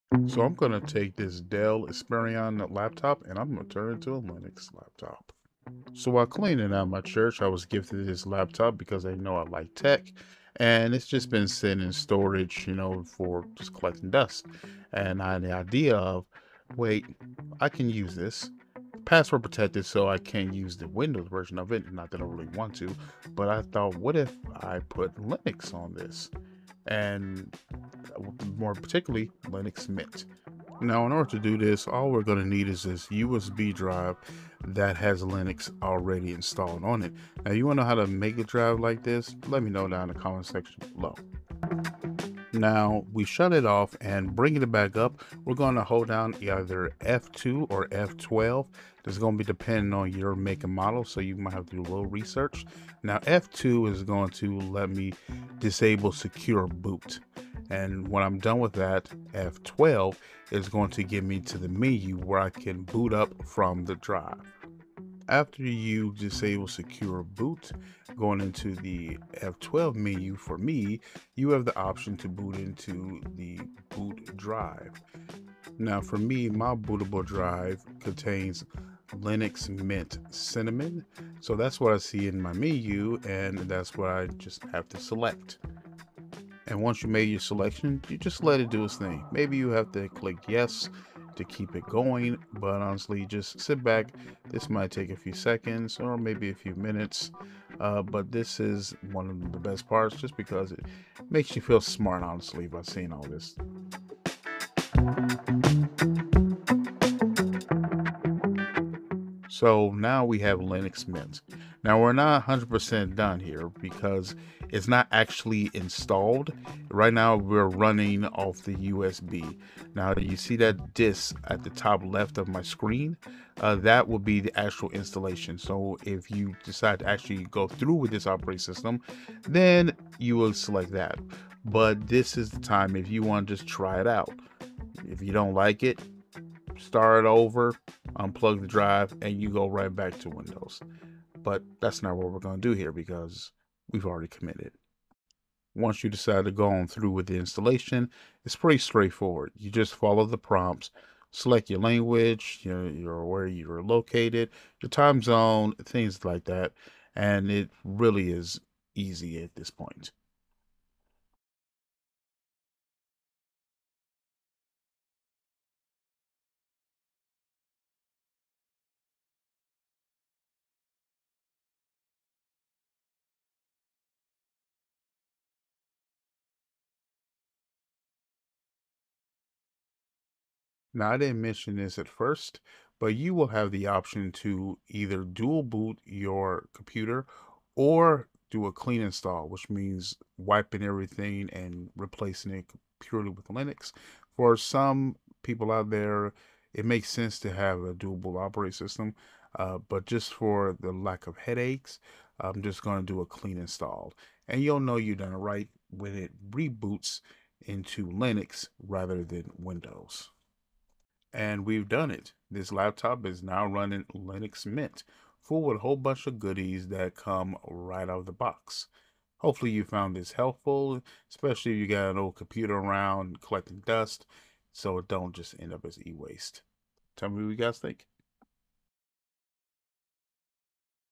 My first Linux laptop
Forgive the vertical. , this is taken from two of my short videos. I installed Linux Mint on a laptop for the first time.t